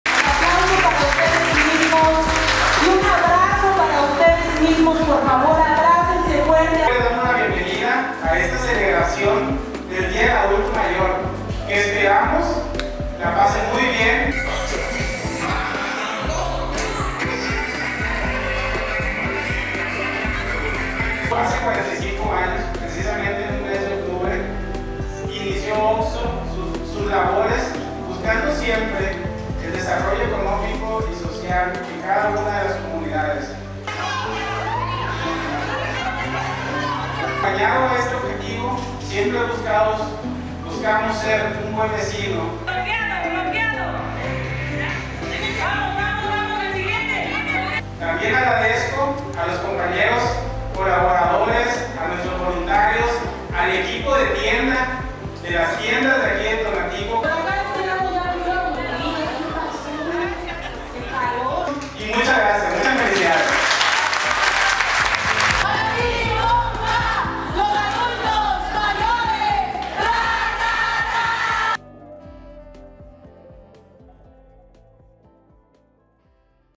Gran festejo de la Cadena Oxxo por el Día Internacional del Adulto Mayor
Como parte del programa de Responsabilidad Social que viene impulsando la cadena comercial Oxxo, se llevaron a cabo, en coordinación con el DIF municipal de Tonatico, actividades para homenajear e integrar a los adultos mayores de la localidad.